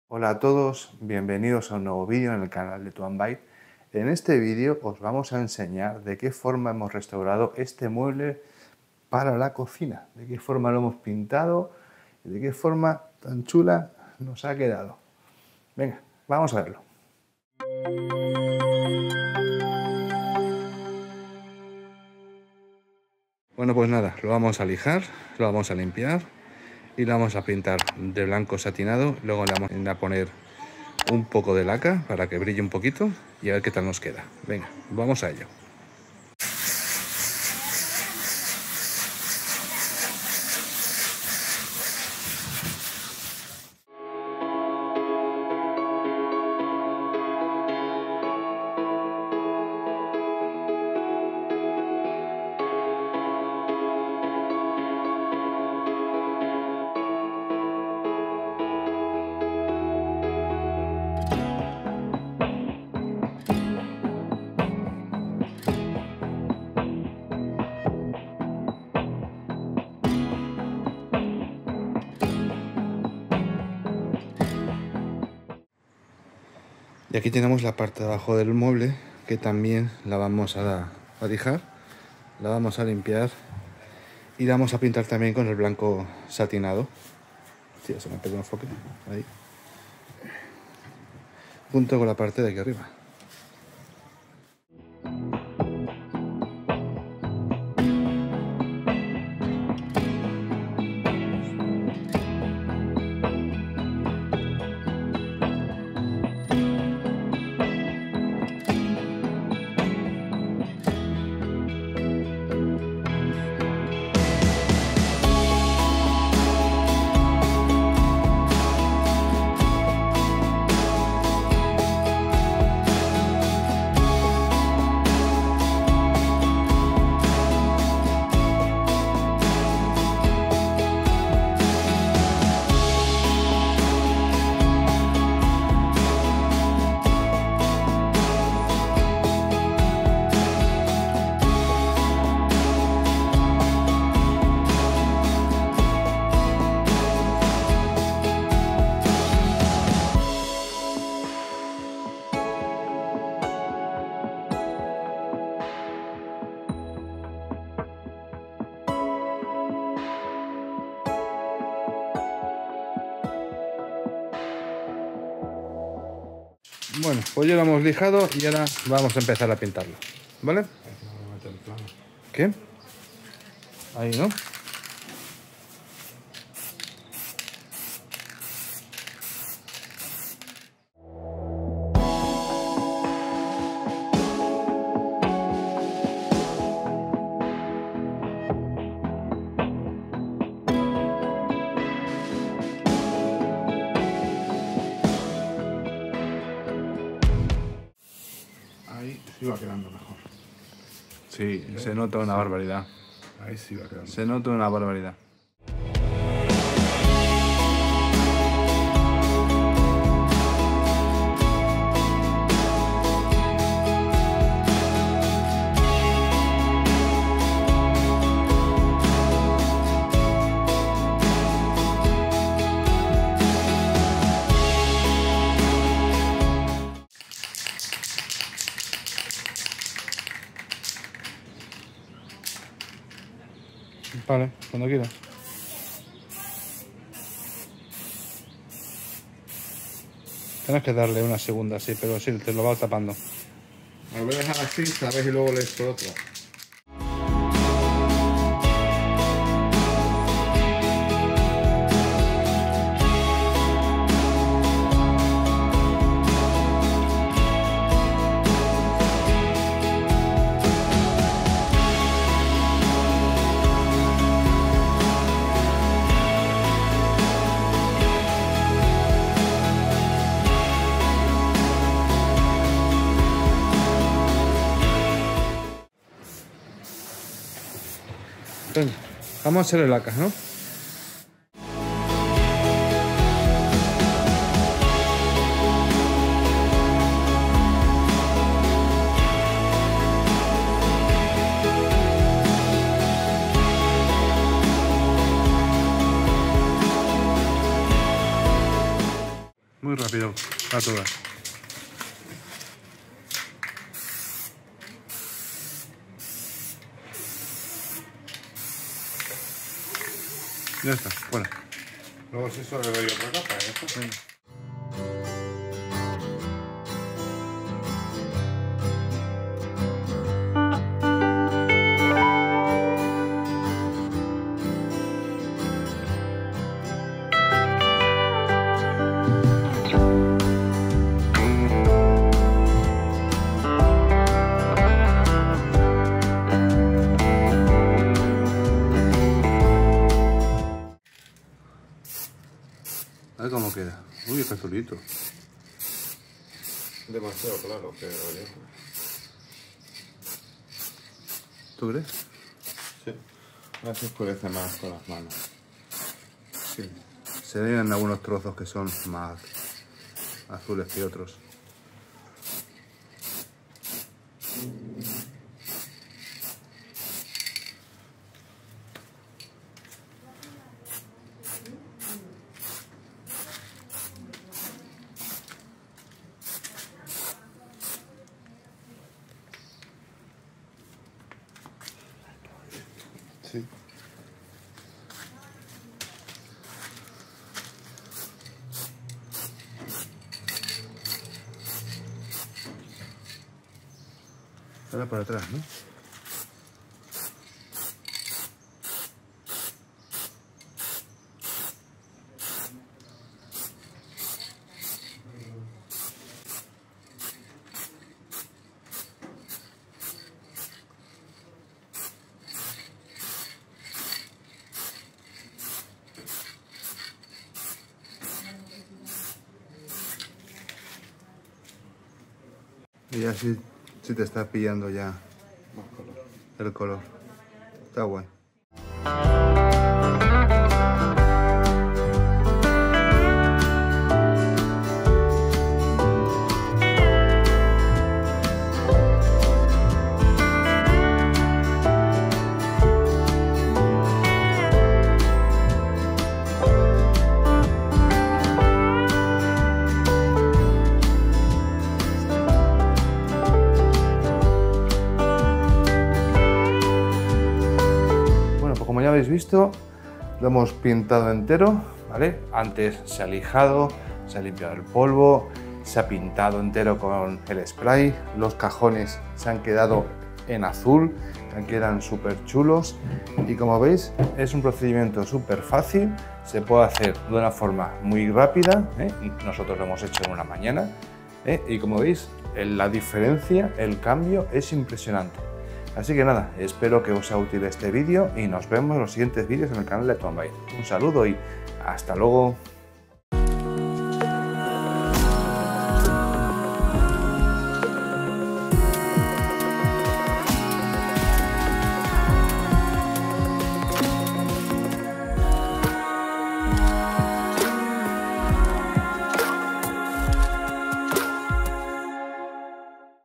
Restauración épica de un mueble de cocina deteriorado: ¡Te sorprenderá el resultado!
En este vídeo restauramos un mueble para la cocina del chalet.

Saludos!